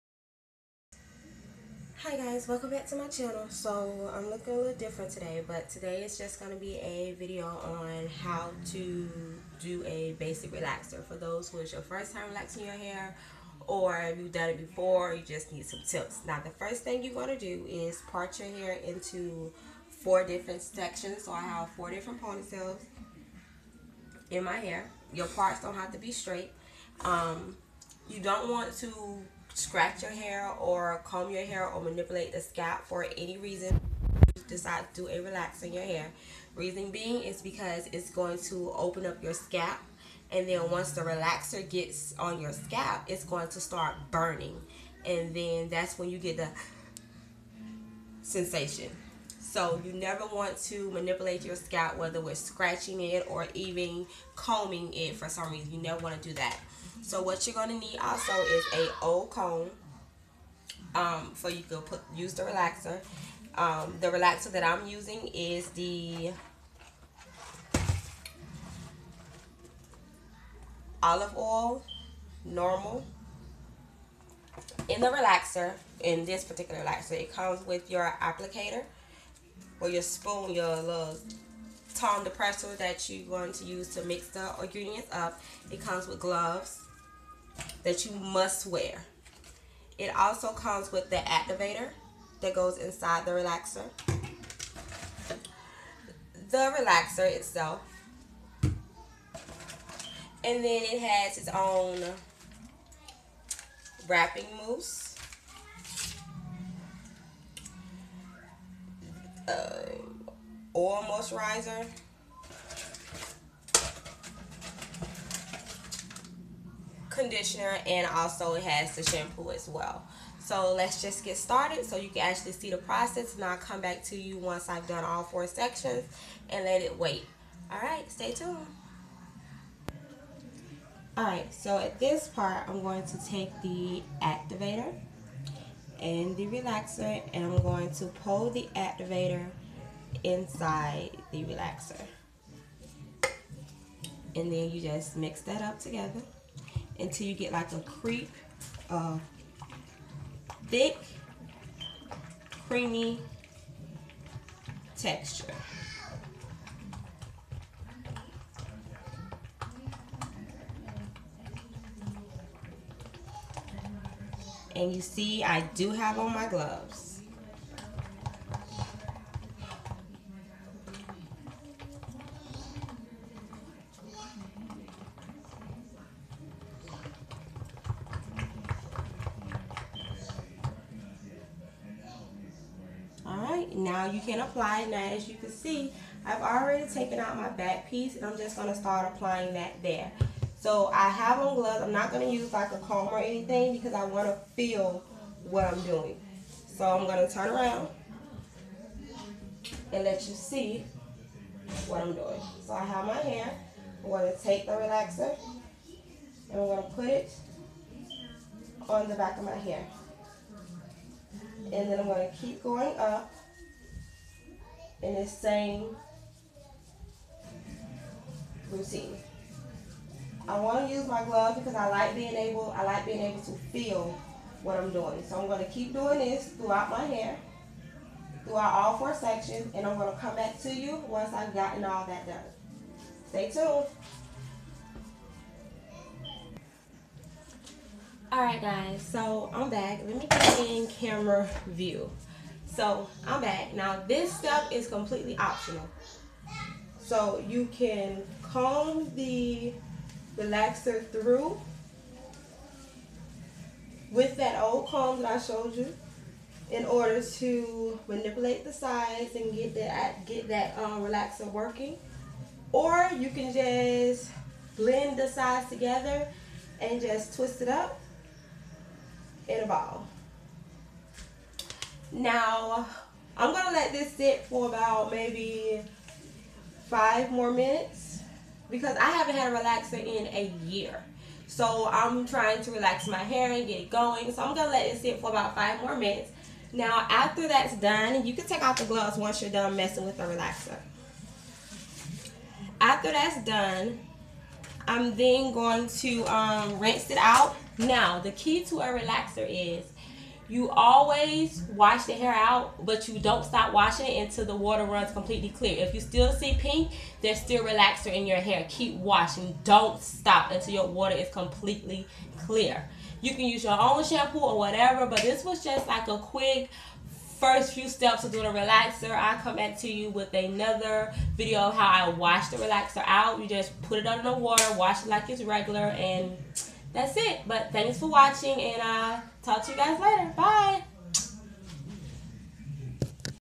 How to relax your hair
This is a video off how I relax my hair.. I am 1 year post relaxer and decided to start the year off with a relaxer... hope you enjoy